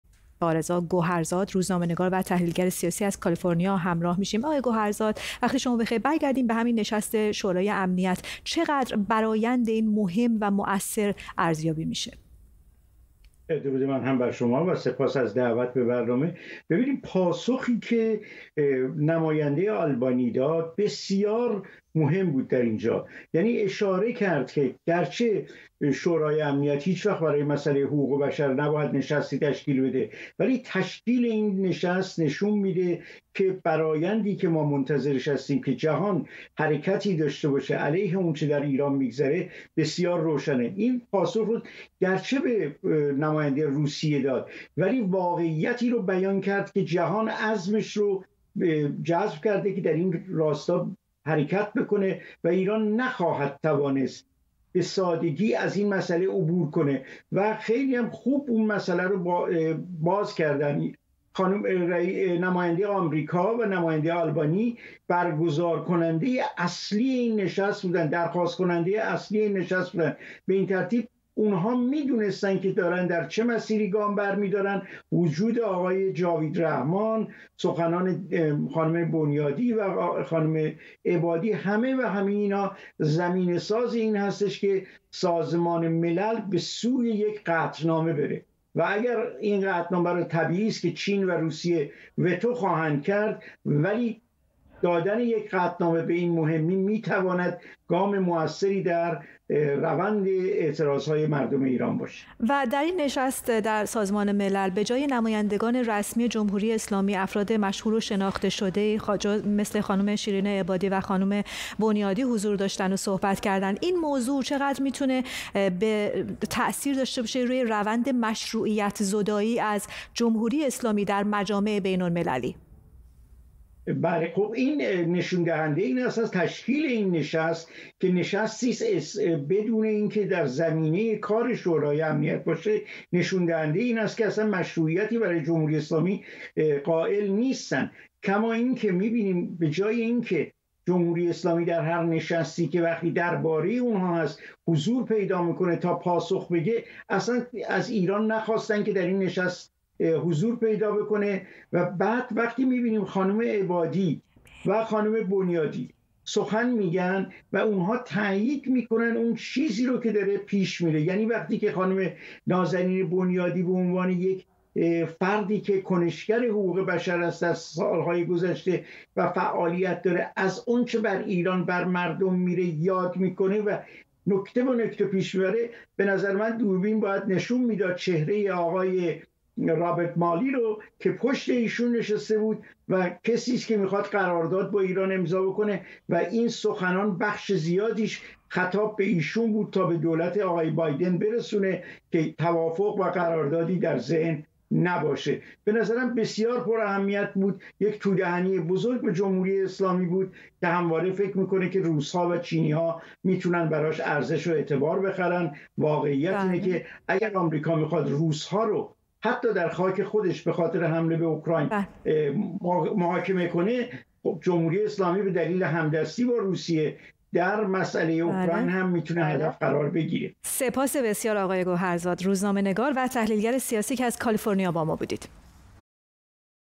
ارزیابی رضا گوهرزاد تحلیلگر سیاسی از نشست شورای امنیت سازمان ملل درباره خیزش انقلابی مردم ایران
ارزیابی رضا گوهرزاد، روزنامه‌نگار و تحلیلگر سیاسی از نشست شورای امنیت سازمان ملل درباره خیزش انقلابی مردم ایران علیه جمهوری اسلامی